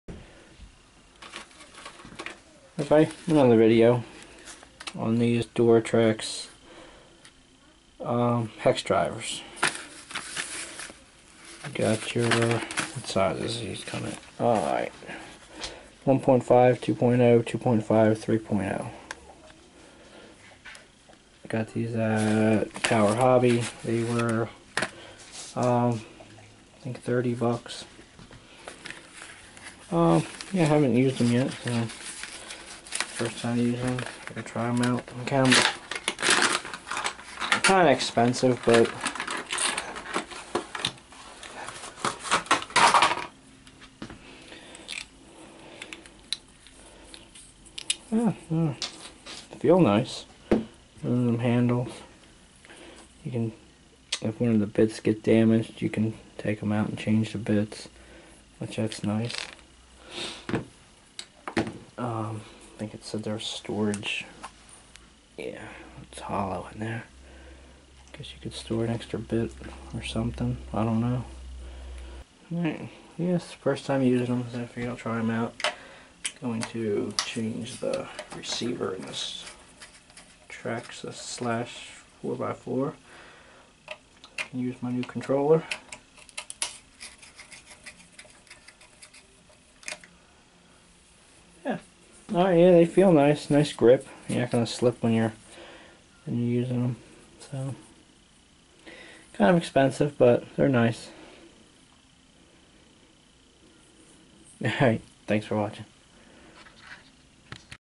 Duratrax 4-Piece Ultimate Hex Driver Set
Here's a short video review of the Duratrax 4 piece ultimate hex driver set I pick these up at Tower Hobbies for under $30 kind of expensive but the quality is really nice you get a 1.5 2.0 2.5 and 3 .0 one good thing is you can get replacement tips in case you damage one if you're looking for a high-quality hex driver set I think this is what you're looking for thanks for watching if you like this video give it a thumbs up and subscribe